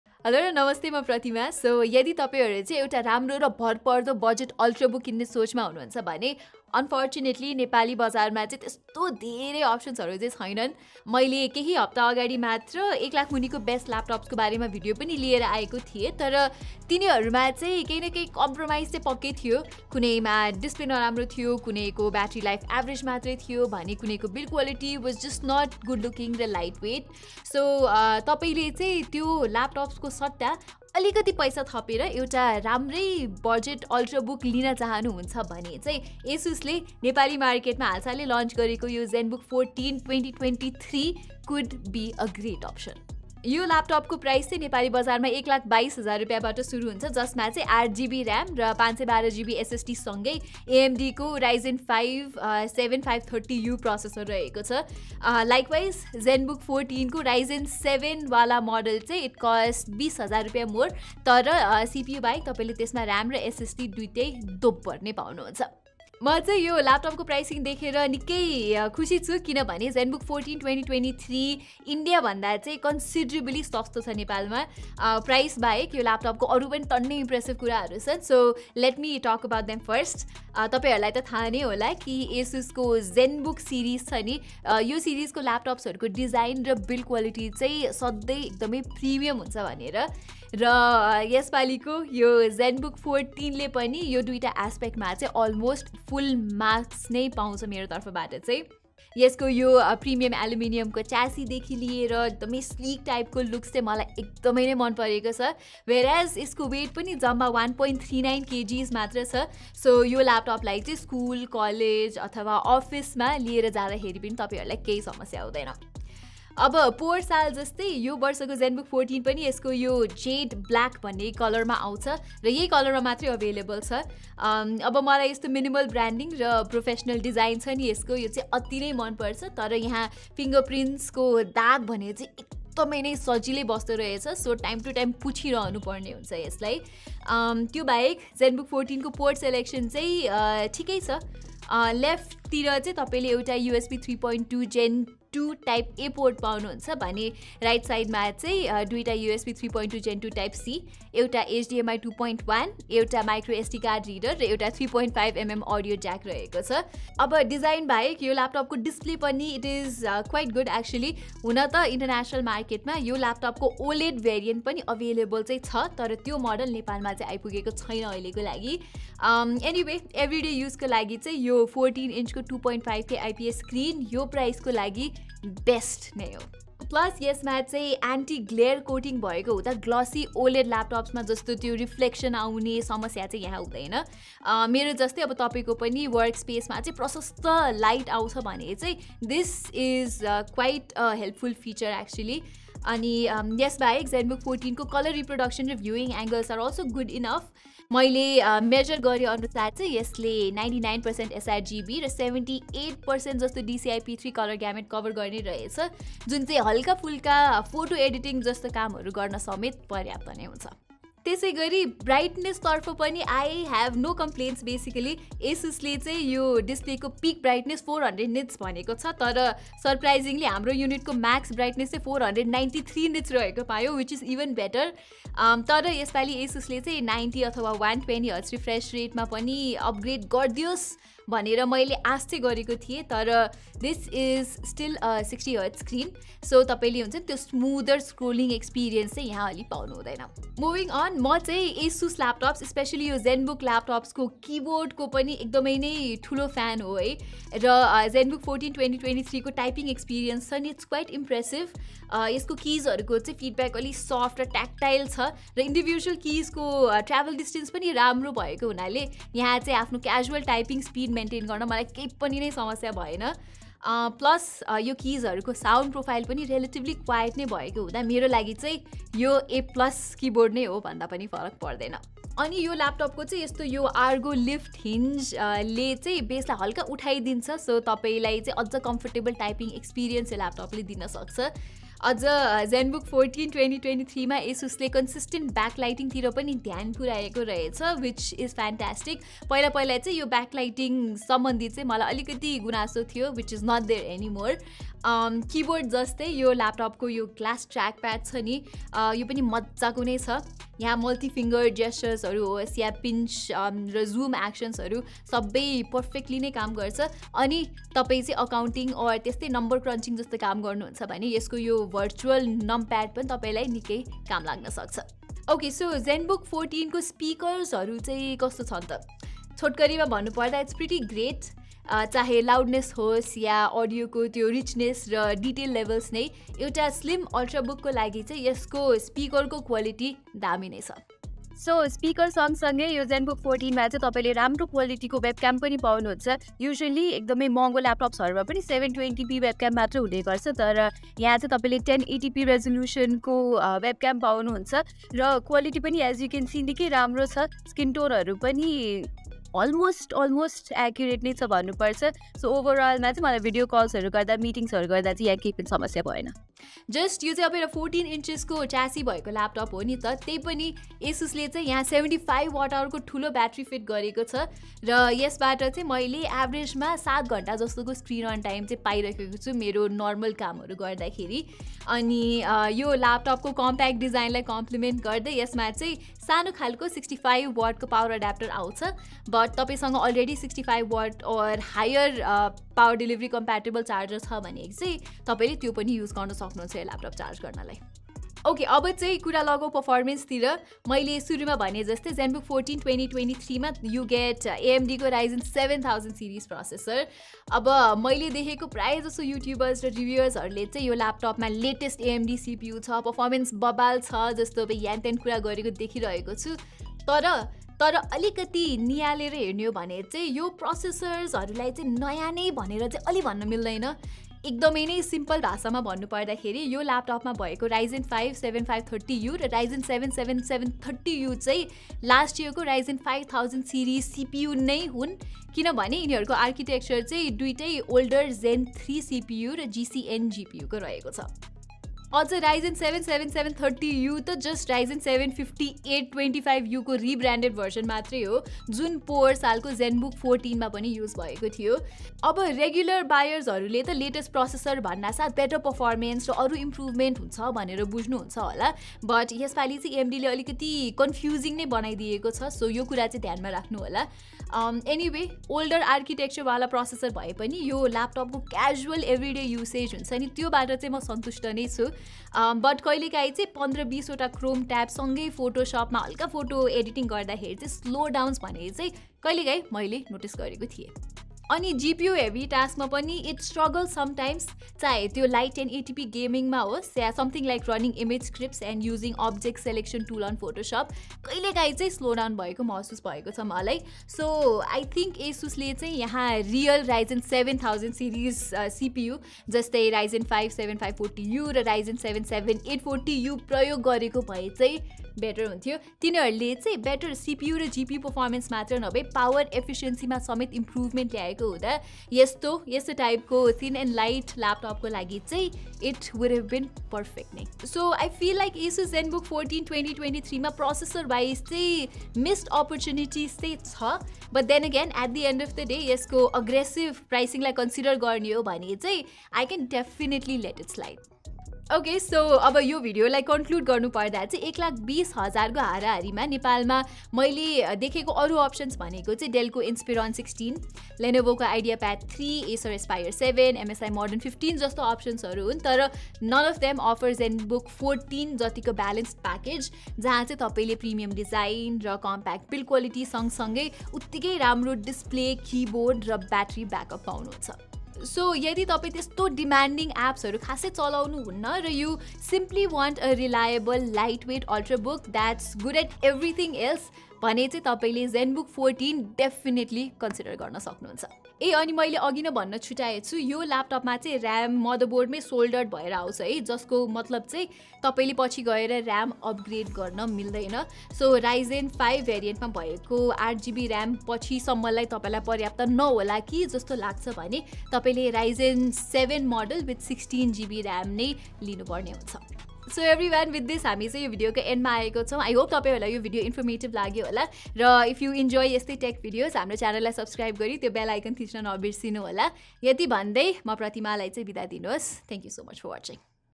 Asus Zenbook 14 (2023) Review: Premium Ultrabook under रु 1.2 Lakhs!
Today, in this video, I am reviewing the new Asus Zenbook 14 (2023) that has been launched for Rs. 1.2 lakhs in Nepal. Should you buy this ultrabook instead of a budget laptop?

[Nagmani International - Asus Distributor for Nepal | 2 years warranty]

Gears we use:
🔹Secondary lens
🔹Microphone for Audio
🔹Lav Mic
🔹Tripod Head